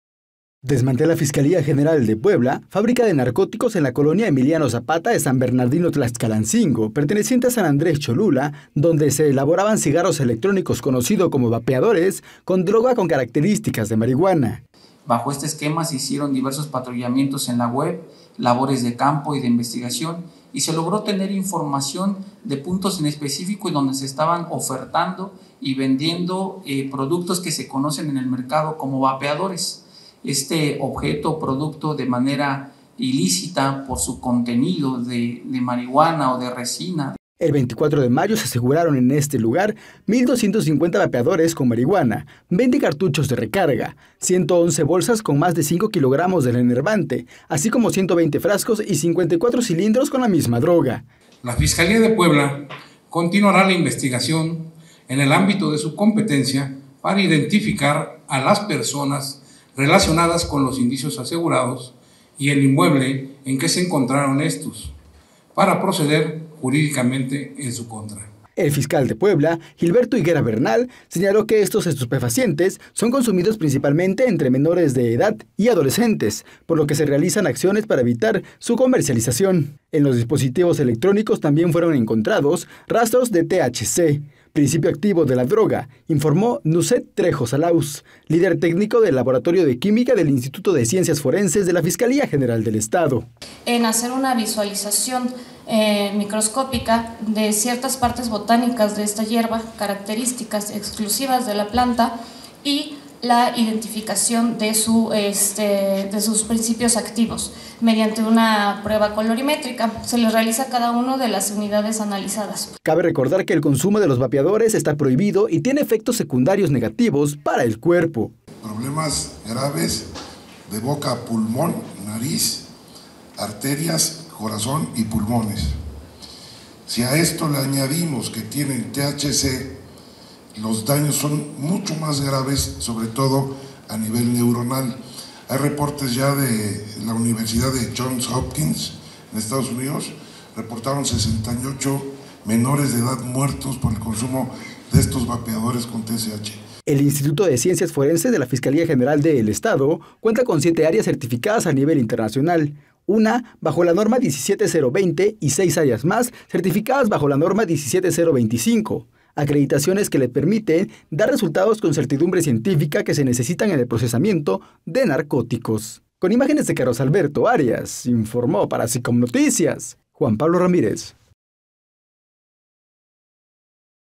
‼️ La FGE desmanteló un "narcolaboratorio" en San Andrés
‼️ La Fiscalía General del Estado de Puebla desmanteló un "narcolaboratorio" en San Andrés Cholula; se elaboraban "vapeadores" con marihuana.

Reportaje: Juan Pablo Ramírez